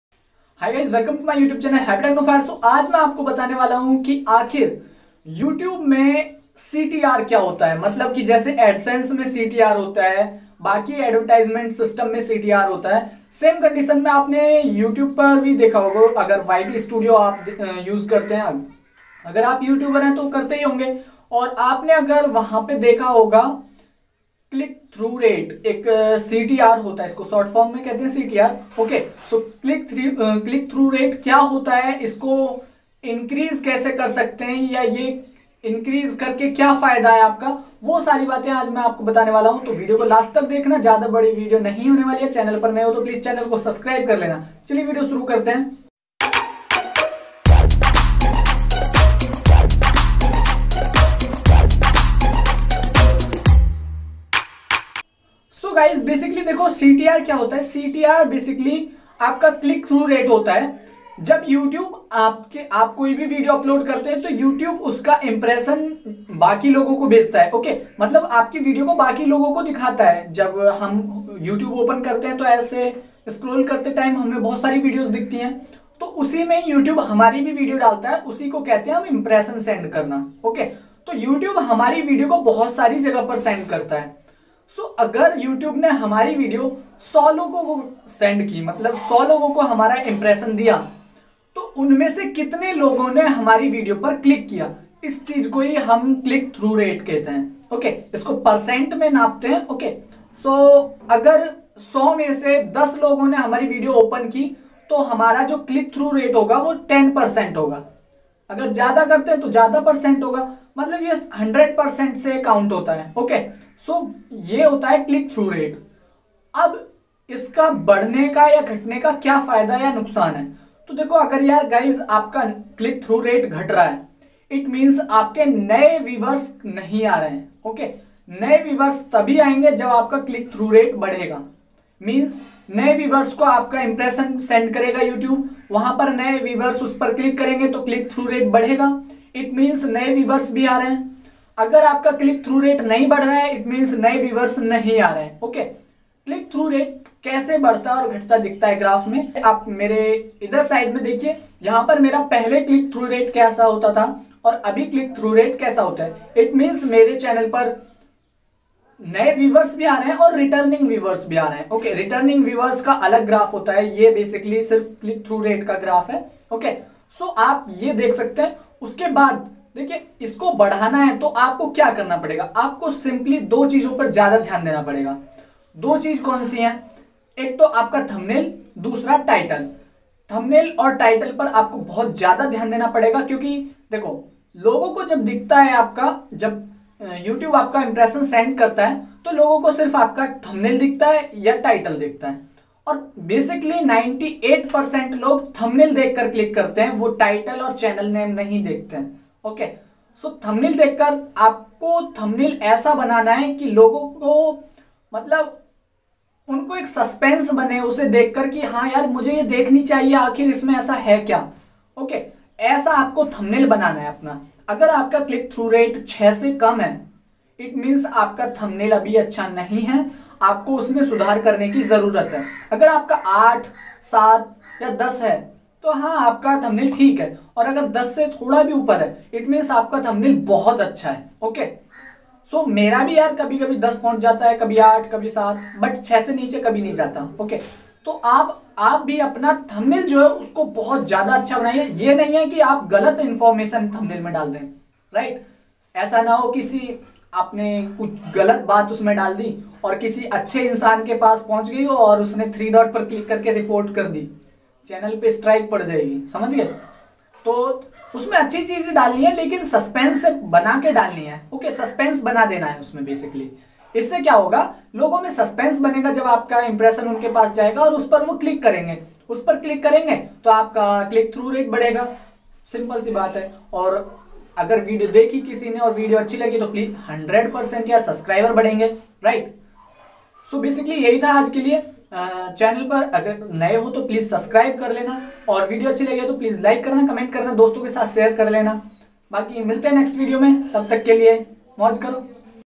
What is CTR in YouTube ? How can you increase it ?
Hey guys , CTR ( Click Through Rate ) kya hota hai YouTube studio mein aur iske kaise badha sakte hain iske badhane se kya fayda hota hai ya iske ghatne se kya nuksan hota hai to vah sari baten discuss ki gai hai video ko last Tak jarur dekh na ho sake to doston ke sath share krna .

what is click through rate.
how to increase click through rate.
what is benefit of increasing click through rate.
channel par nye viewers kaise bdhaye.